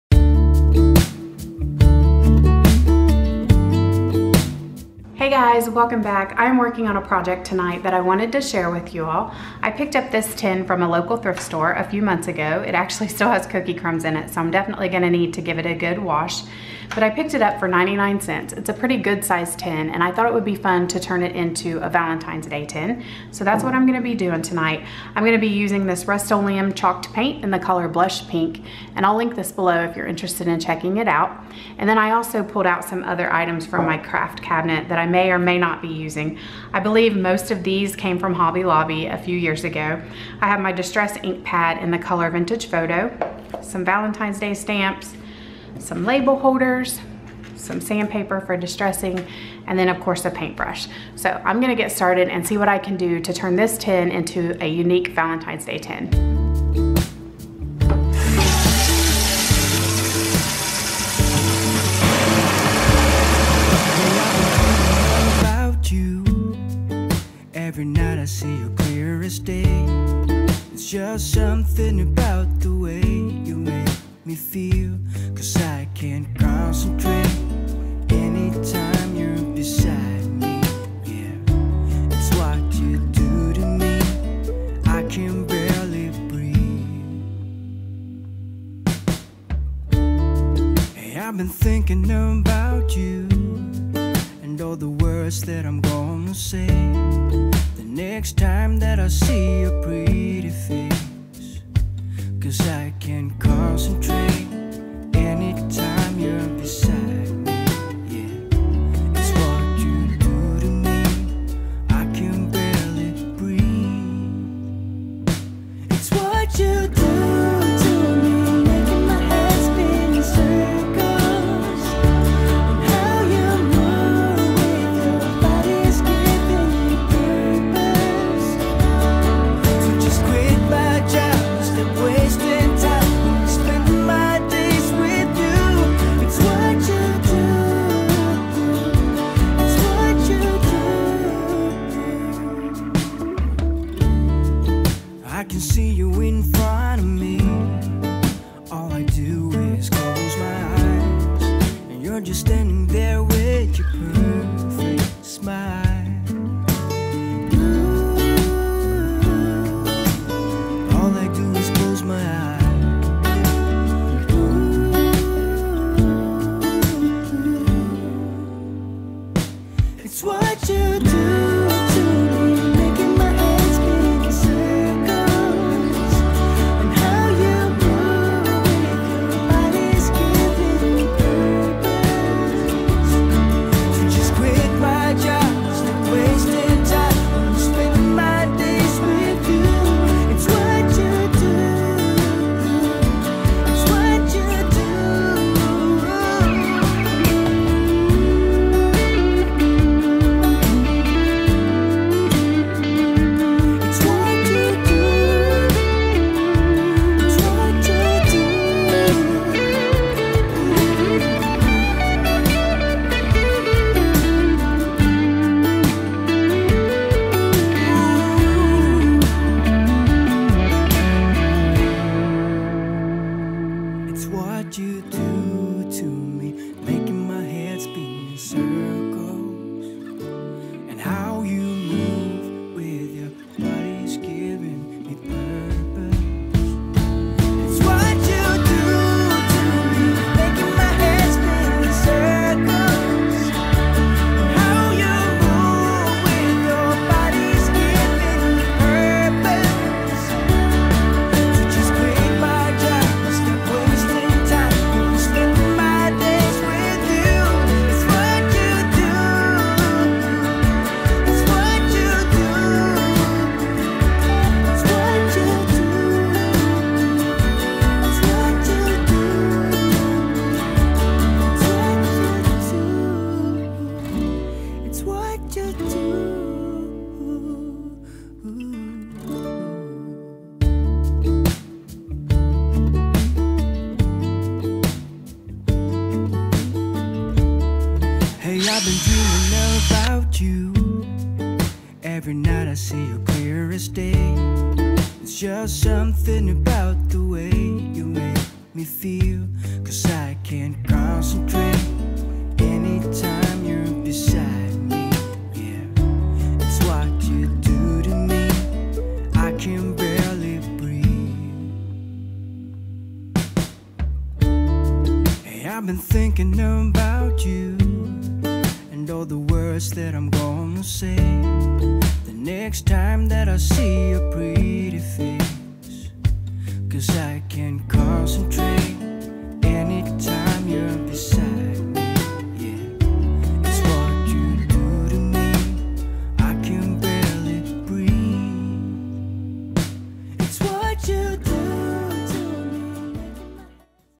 Vintage Valentines Day Tin | Thrift Store Makeover
Join me as I makeover this thrift store find to create a vintage style Valentine's Day tin.

Items to create your own Vintage Style Tin:

Chalked Paint (Blush Pink):

Metal Label Holders:

Distressed Ink Pad (Vintage Photo):

Heart Stamps:

(I couldn't find the mini heart stamp I used, so I linked similar stamps)

Items Used to Create Video:
Canon G7x
Adobe Premiere Pro CC
Acer Predator 17 Gaming Laptop

251 W. Central Avenue #227
Springboro, OH 45066